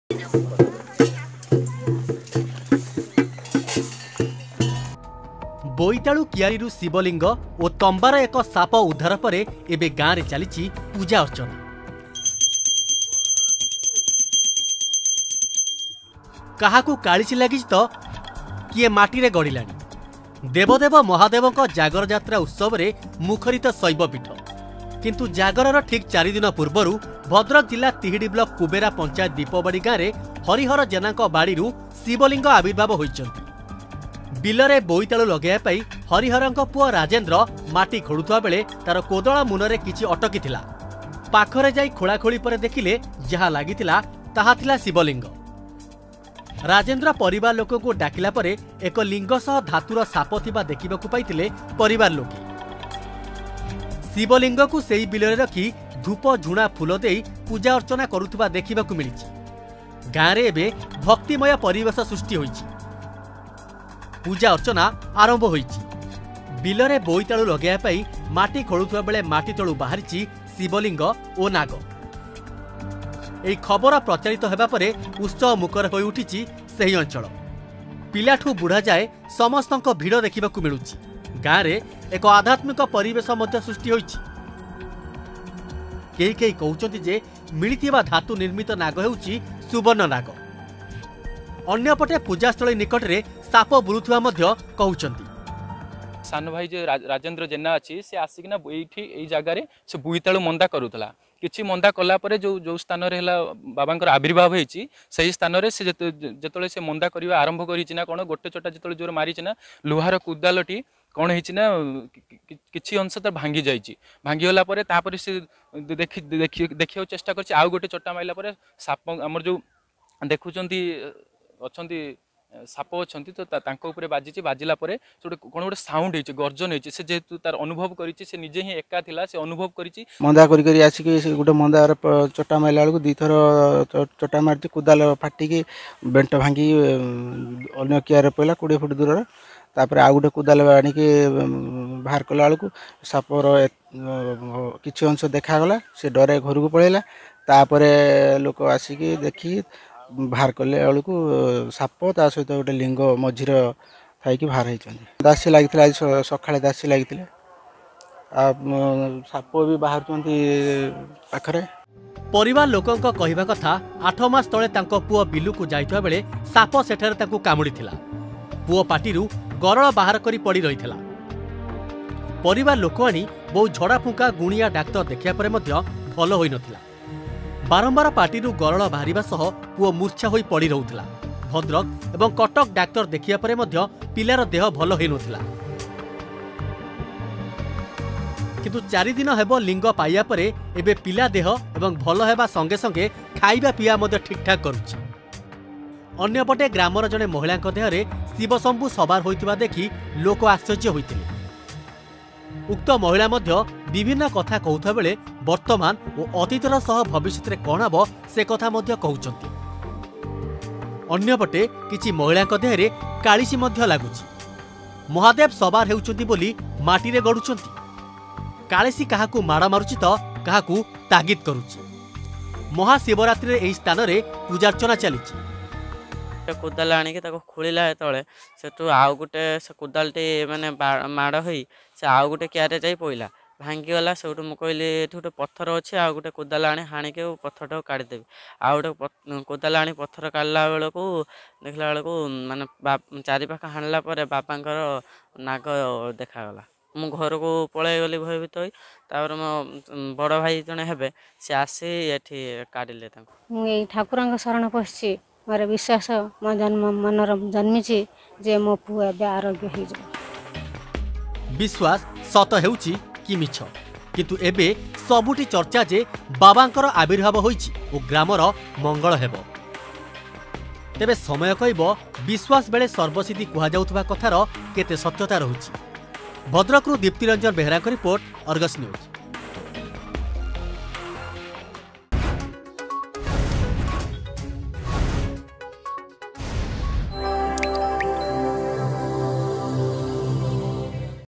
ମାଟି ଖୋଳୁ ଖୋଳୁ ମହାଦେବ କଥାକୁ ସତ ମାନିଛନ୍ତି ଗାଁ ଲୋକେ...
ମାଟି ଖୋଳୁ ଖୋଳୁ ମହାଦେବ କଥାକୁ ସତ ମାନିଛନ୍ତି ଗାଁ ଲୋକେ । ବୋଇତାଳୁ ଚାଷ ପାଇଁ ମାଟି ଖୋଲିବା ବେଳେ କୋଦାଳ ଭାଙ୍ଗିଗଲା । ଶେଷରେ ଗାଁ ଲୋକେ ଖୋଲିବା ବେଳ ଏକ ଶିବ ଲିଙ୍ଗ ଆକୃତିର ପଥର ବାହାରିଲା ଓ ପରେ ବାହାରିଲା ଏକ ଧାତୁ ନିର୍ମିତ ନାଗ । ଗ୍ରାମ ଲୋକେ ପୂଜା ଆରମ୍ଭ କରିବାମତେ ପୁଣି କାଳେସୀ ଲାଗିଲା ଓ କାଳିସୀ କହୁଛି ମାଟି ଖୋଳ ଆହୁରି ଠାକୁର ସାମଗ୍ରୀ ବାହାରିବ । ଭଦ୍ରକ ଜିଲ୍ଲା କୁବେରା ଗାଁରୁ ଏହି ବିଶ୍ୱାସ ଓ ଅବିଶ୍ଵାସର କଥା ସାମ୍ନାକୁ ଆସିଛି.